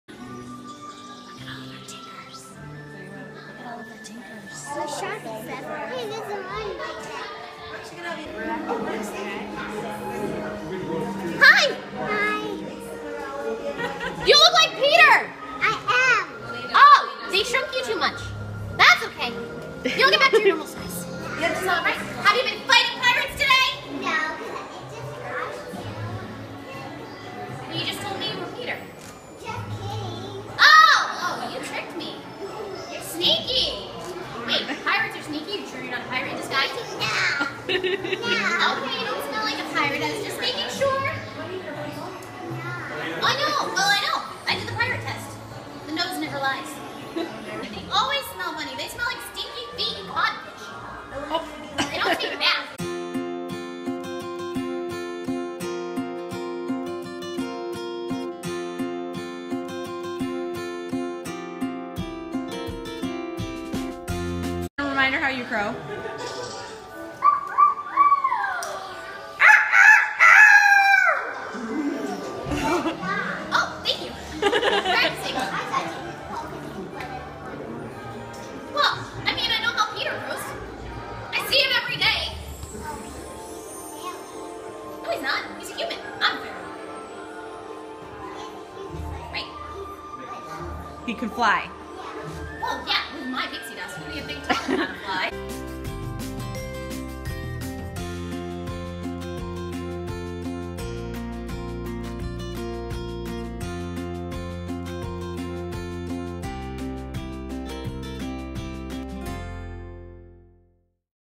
Little Peter Pan meeting his Tinker Bell
Our 2015 trip had many sweet meet and greets. Peter was thrilled to meet his Tinker Bell. She was sassy (as was Peter). Who is your favorite meet and greet?